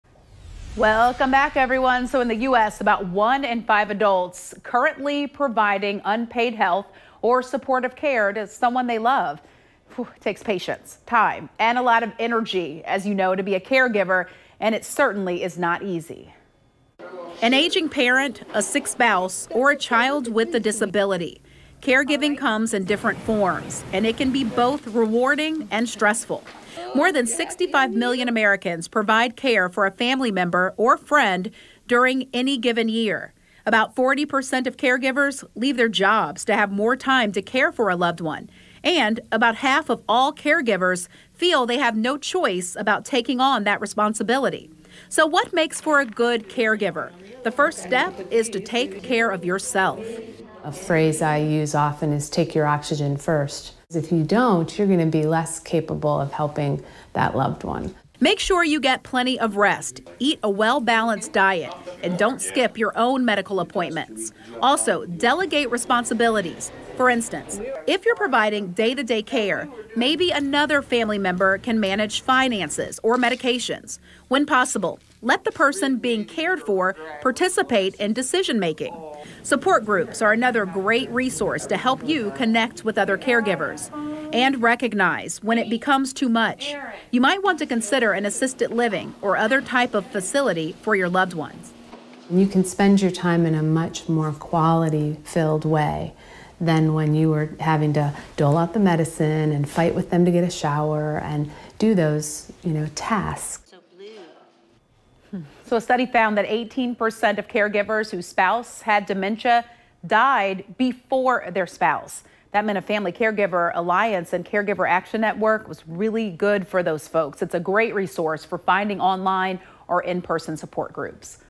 Being a Good Caregiver
Being a caregiver takes patience, time, and energy. About one in five adults in the U.S. are currently providing unpaid health or supportive care to someone they love.